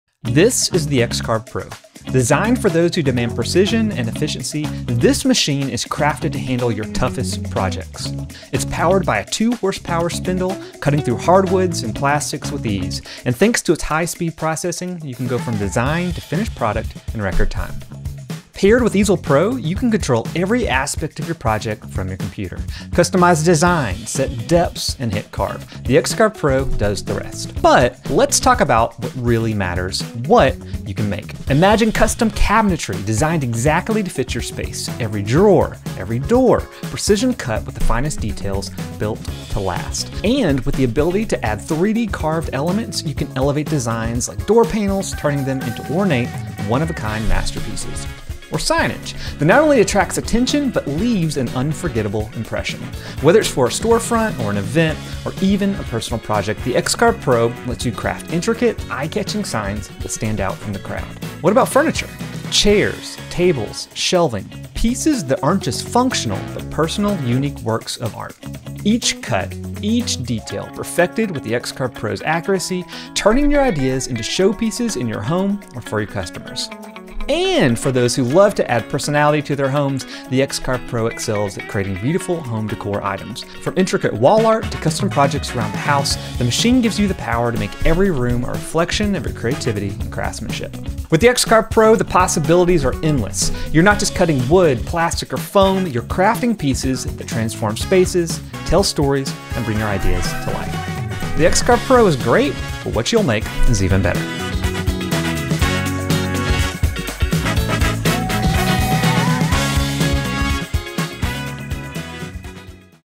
Projects on the X-CARVE Pro
Offered in both 4 x 2 and 4 x 4 foot sizes check out the wide range of projects you can make with the X-Carve Pro CNC.  Bring your ideas to life with easy to use, pro level CNC macahines for any size shop.